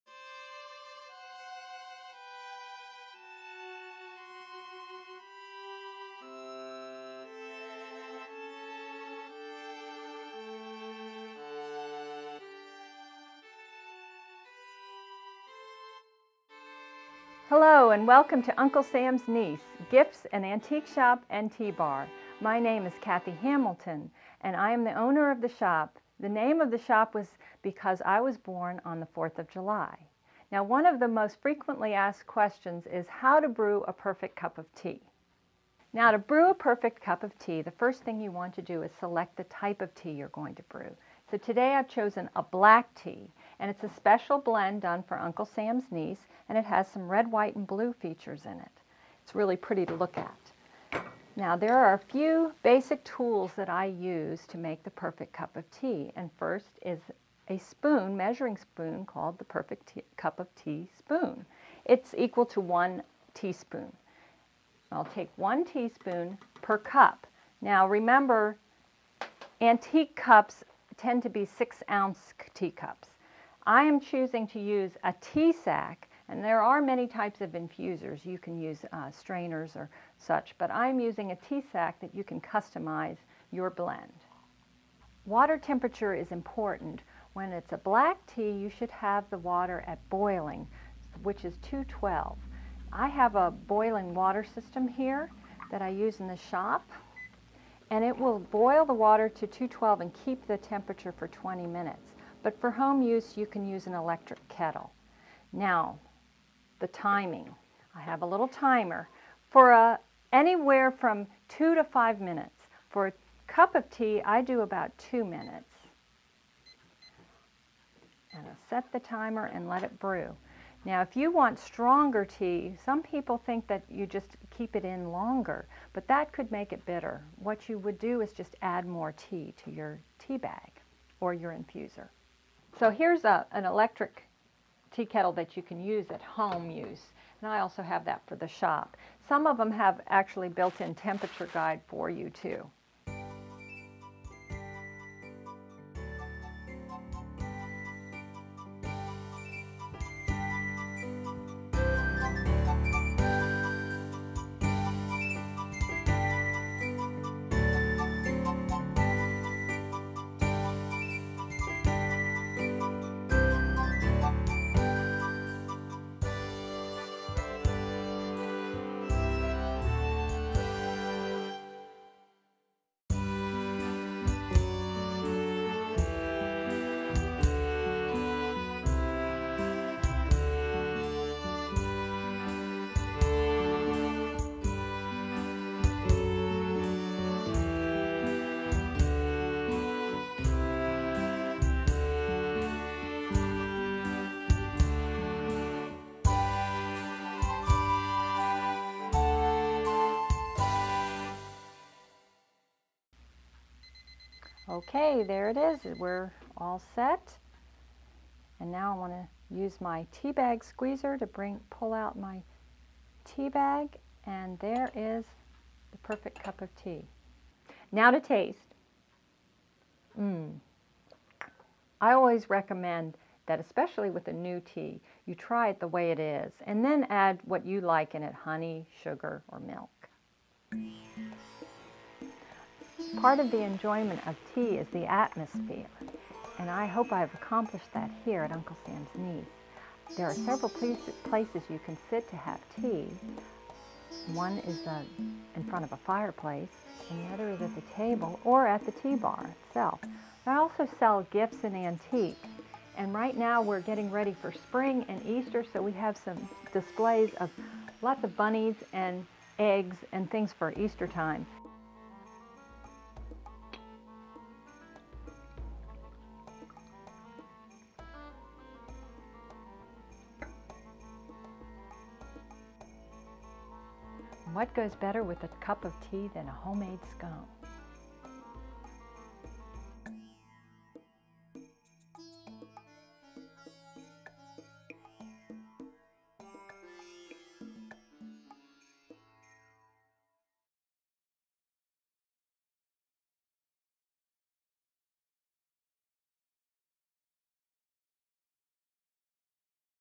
How to Brew a Perfect Cup of Tea - by Uncle Sam's Niece
Uncle Sam's Niece shares how to make the perfect cup of tea.